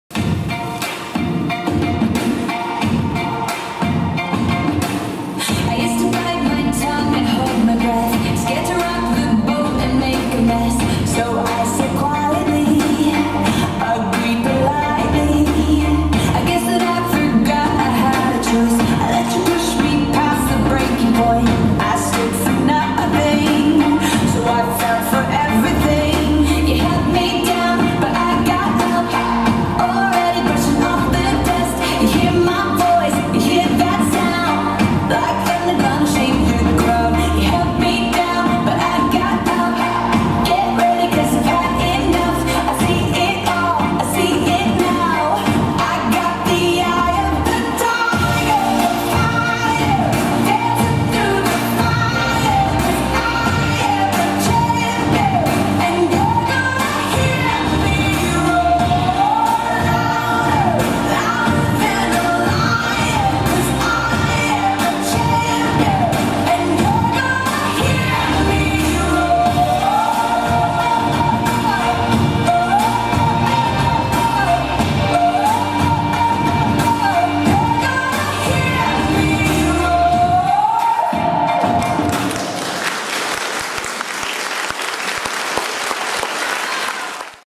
"ПРИНЦЕССА И ТИГР" Хастл в Красноярске на дне рождения ТК "Чердак"
Номер от преподавателей ТК "Чердак" Северин Алексей и Стрельченко Мария